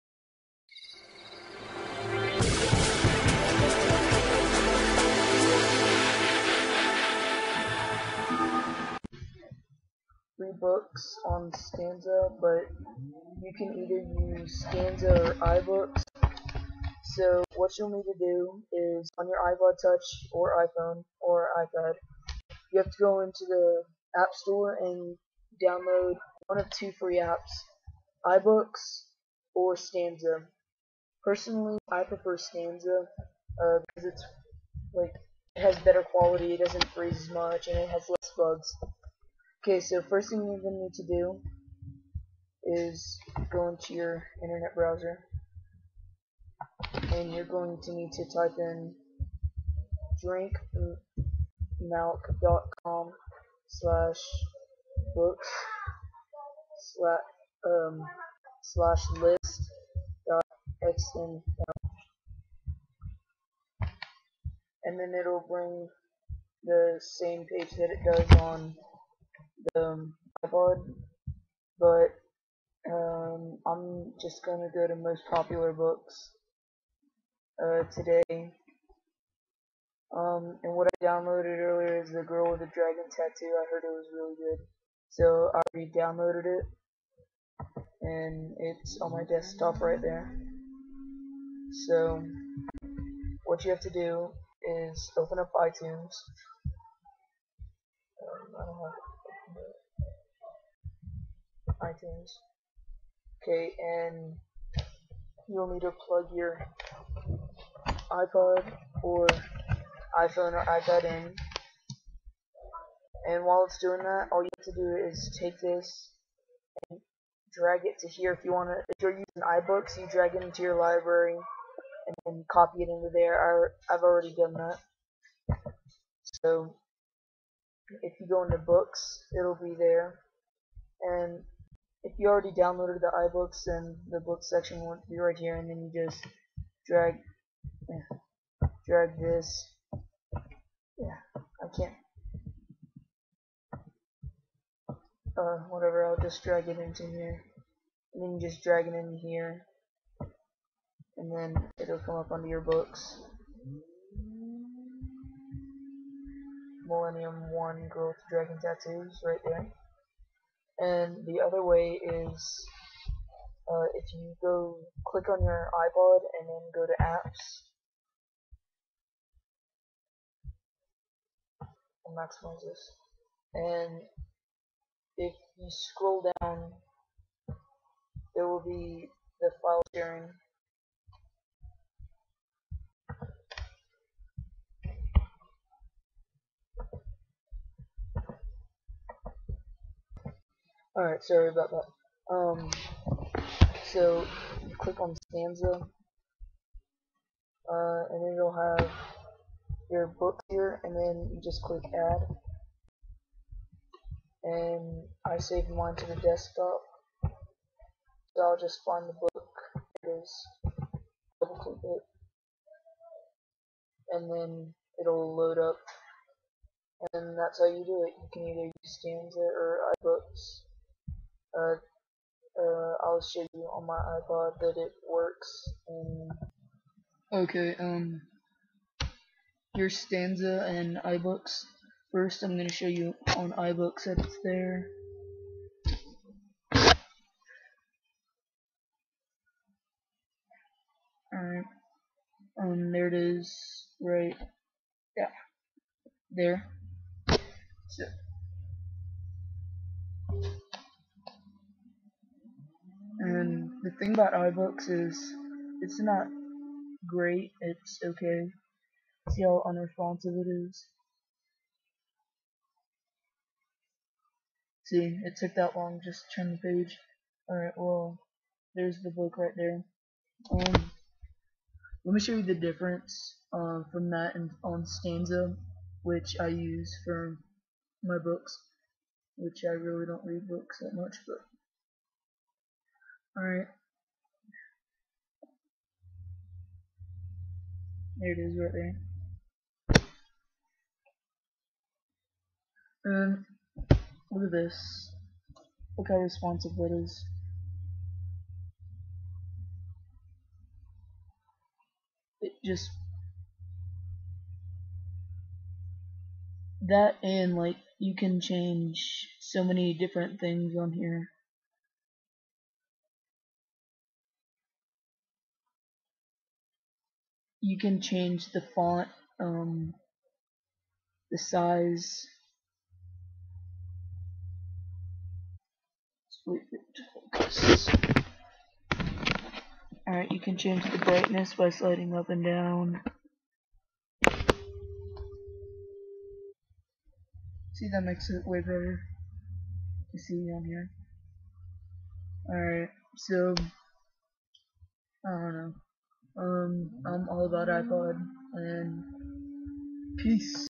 how to put free books on ipod touch with a computer
if you have troubles doing my other tutorial without a computer then this way is almost just as easy